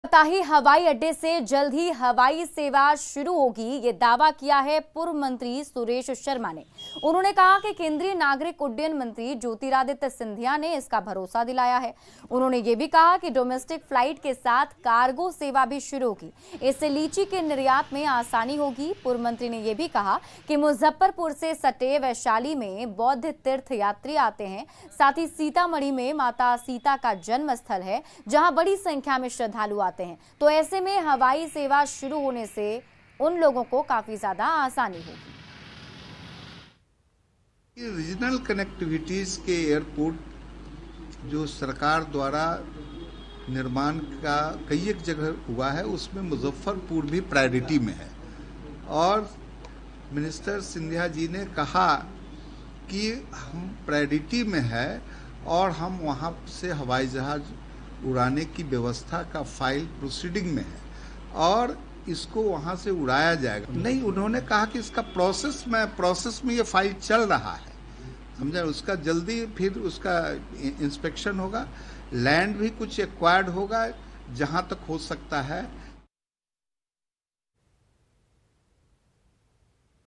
Muzaffarpur: पताही हवाई अड्डे पर शुरू होगी हवाई सेवा, पूर्व मंत्री Suresh Sharma ने किया दावा
Patahi हवाई अड्डे पर जल्द शुरू होगी हवाई सेवा, पूर्व मंत्री Suresh Sharma ने किया इस बात का दावा | Domestic और Cargo सेवा की होगी शुरुआत
bihar news live |aaj ki taza khabar | latest News | hindi News | Latest Hindi News|
| Hindi Samachar | news in hindi | news18 | n18oc_Bihar